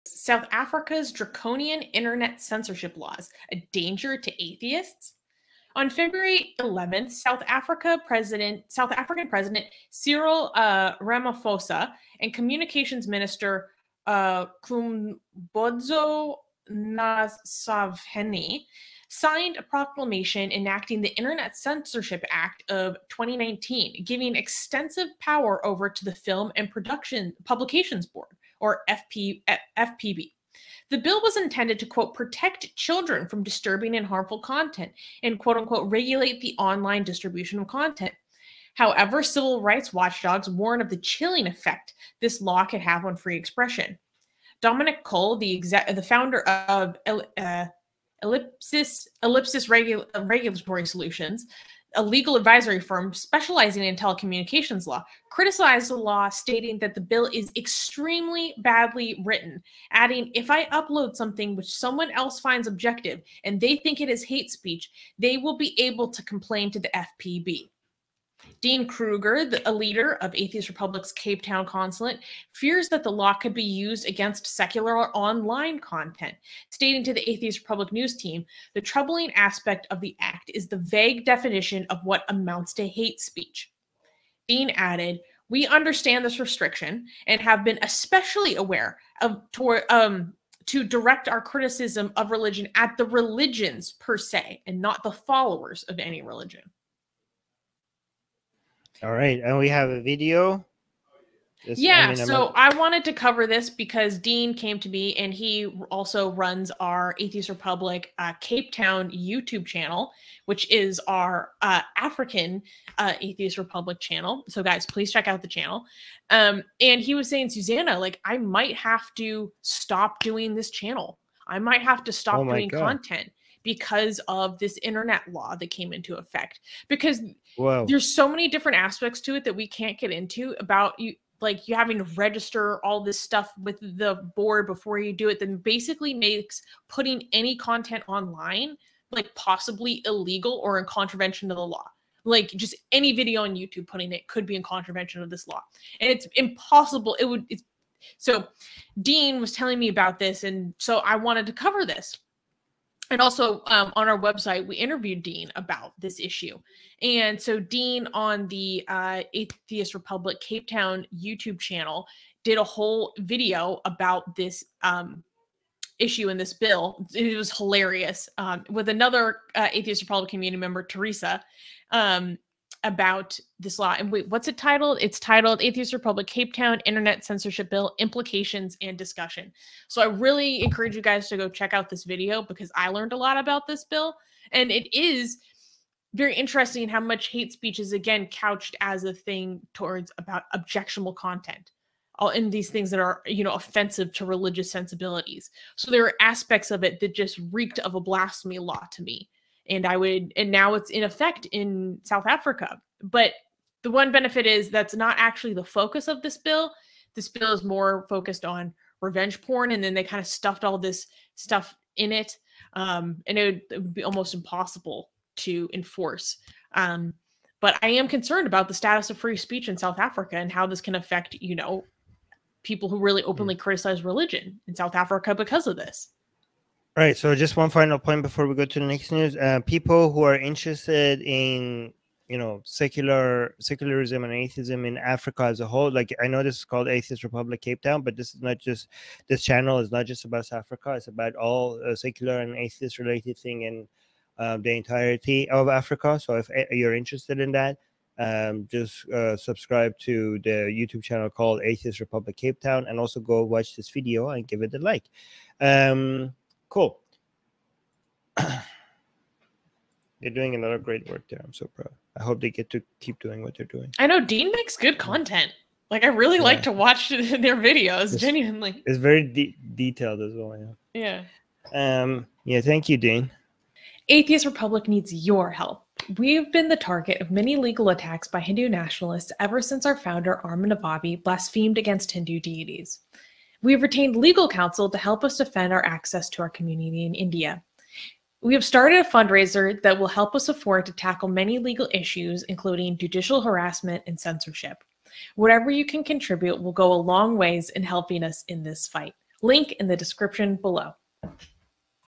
South Africa's Draconian Internet Censorship Laws: A Danger To Atheists?
On February 11th, South African President Cyril Ramaphosa and Communications Minister Khumbudzo Nthsavheni signed a proclamation enacting the Internet Censorship Act of 2019, giving extensive censorship power to the Film and Publications Board (FPB). The bill was intended to "protect children from disturbing and harmful content" and "regulate the online distribution of content." However, civil rights watchdogs warn of the chilling effect this law could have on free expression. Dominic Cull, the founder of Ellipsis Regulatory Solutions, a legal advisory firm specializing in telecommunications law, criticized the law stating that the bill is "extremely badly written," adding “If I upload something which someone else finds objectionable, and they think it hates speech, they will be able to complain to the FPB.” Dean Kruger, a leader of Atheist Republic‘s Cape Town Consulate, fears that the law could be used against secular online content, stating to the Atheist Republic News Team, “the troubling aspect of the act is the vague definition of what amounts to hate speech.” Dean added, “We understand this restriction and have been especially aware of directing our criticism of religion at the religions, per se, and not the followers of any religion.”